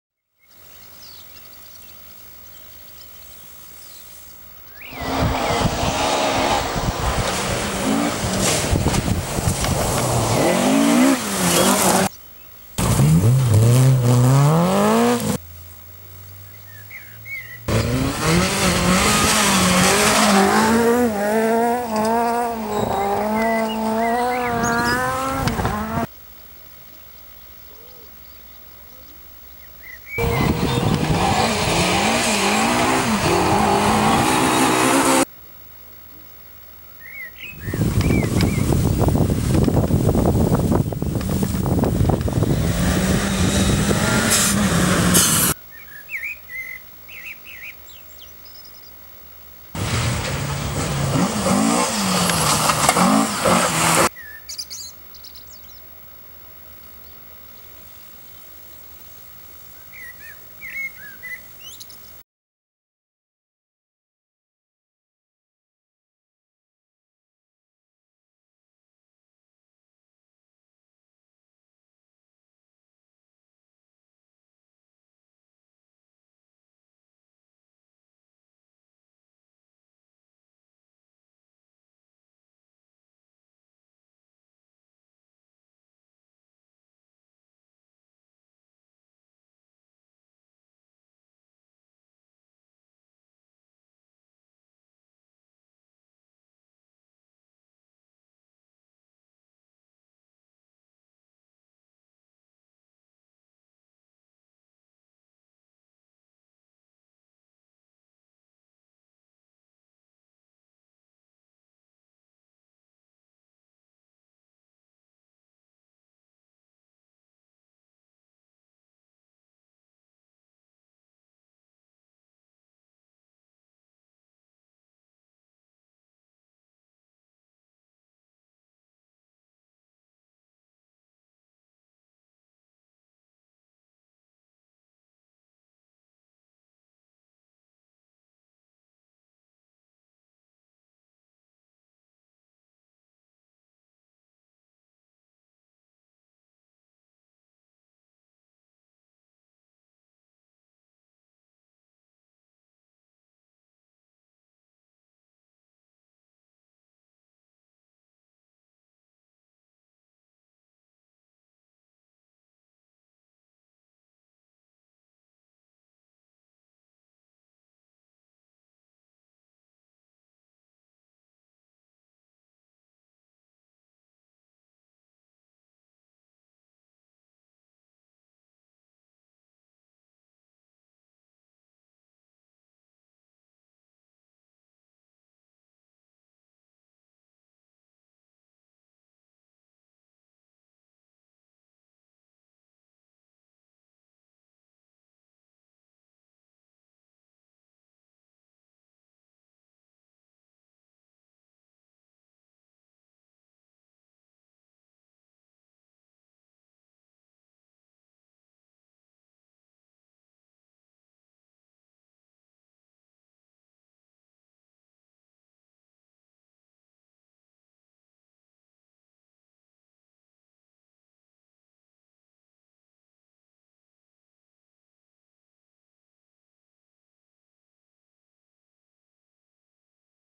Danish Rally Championship 2013 in Skov Oleson Plantage
Muddy ground, wet leaves + gravel = Great driving conditions in Hauge, Denmark. Oktober 26, 2013